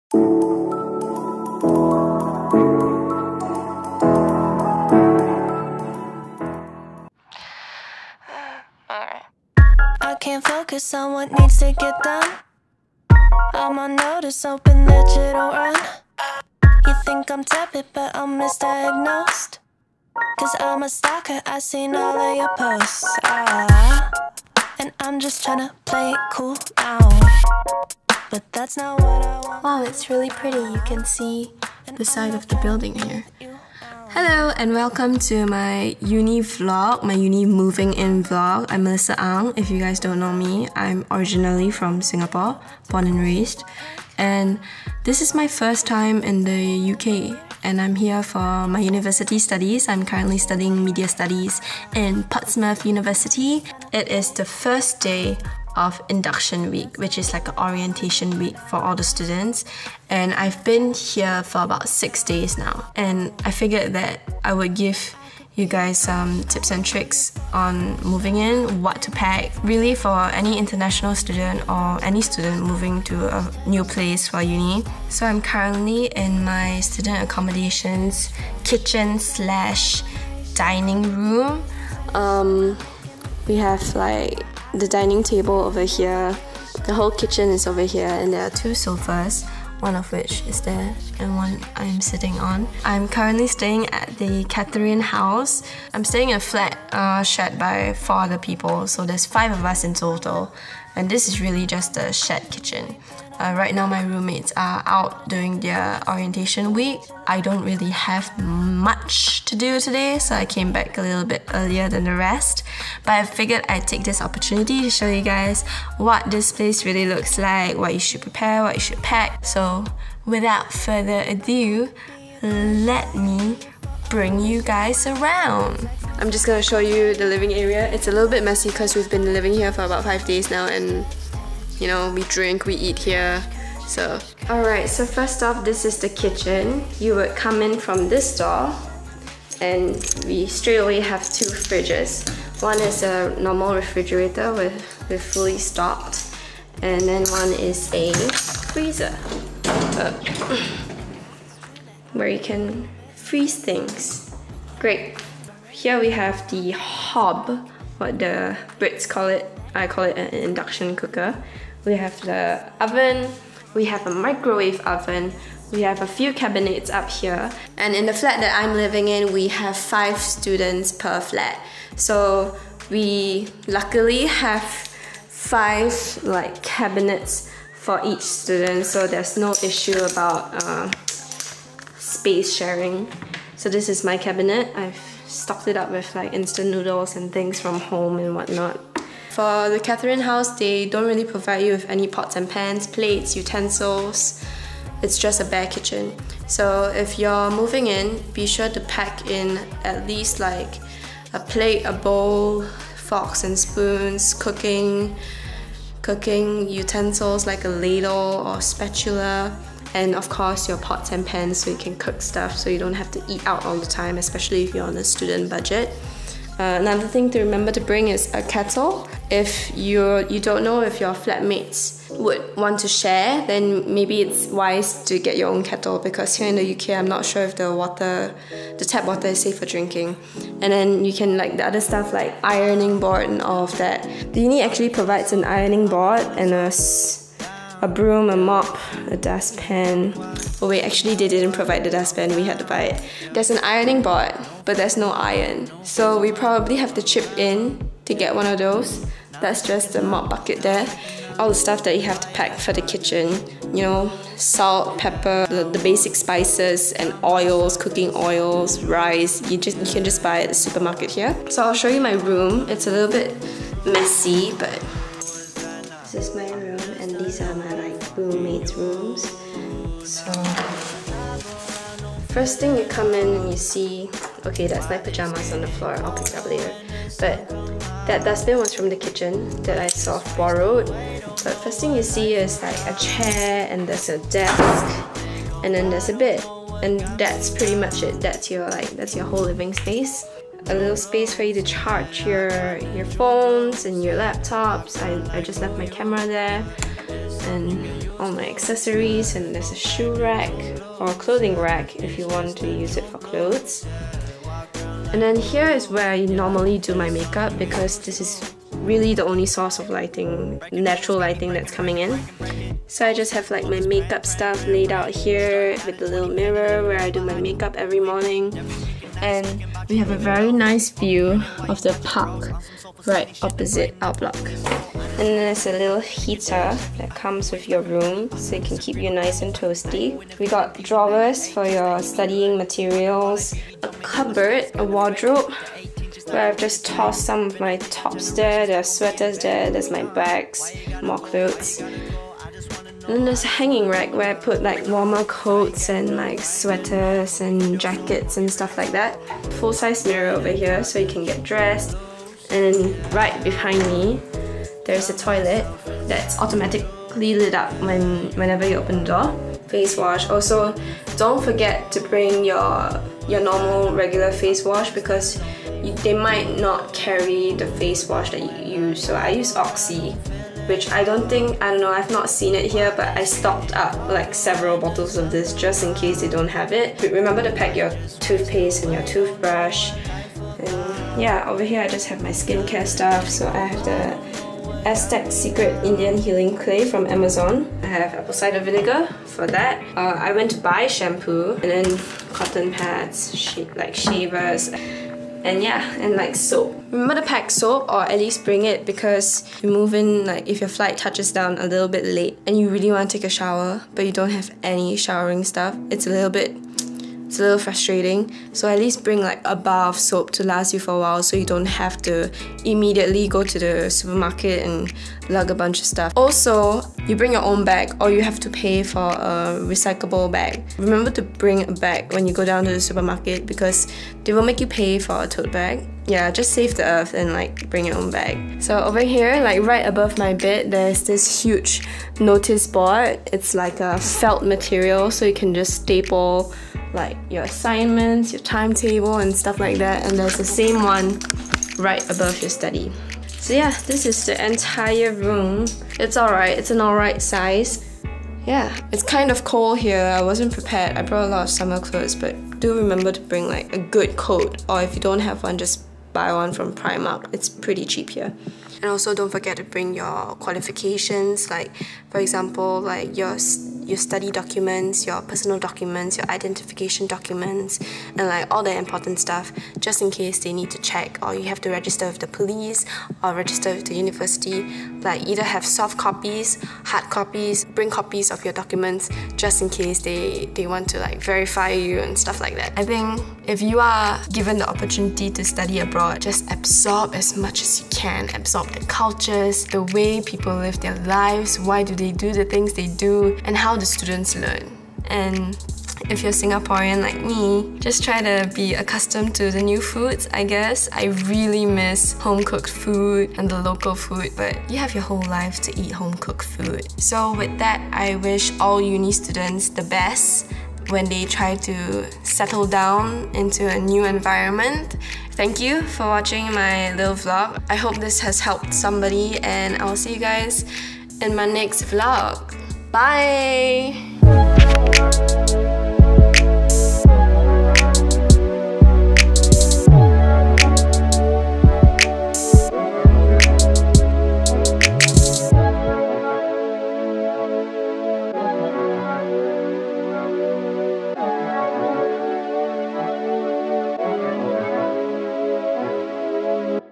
Moving to the UK for University Vlog
hello friends :)

It's been about 2 weeks since I've left Singapore for my university studies in the UK and I thought I'd share my experience + give a tour of the student accommodations here at The Catherine House (just cause when I was about to move here, I was mad researching about what this place looked like).

Also- I have really chapped lips here I know. I have lip balm now, please ignore it.

Word of advice: Pack your asian foods (chillies/ sauces/ spices) in your check-in bags if you can! Depending on your location, it may be difficult to locate an asian food store.

I hope this helps someone prepare for their trip to University in regards on what to pack!